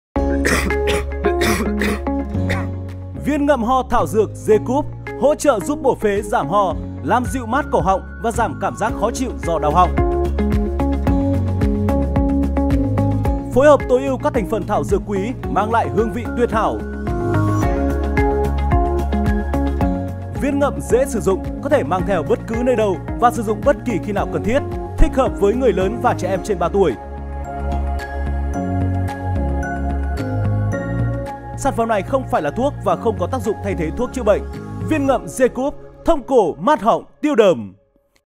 Viên ngậm ho thảo dược Zecuf | Quay video 360 giới thiệu sản phẩm ngành Dược
Dự án quay video 360 giới thiệu sản phẩm Viên ngậm ho thảo dược Zecuf, được thực hiện bởi ShopVideo by ColorStudio - Đơn vị chuyên sản xuất video Marketing dành cho doanh nghiệp vừa và nhỏ.

📌 Địa chỉ: 95 K2 Nguyễn Văn Giáp, Phú Đô, Mỹ Đình, Hà Nội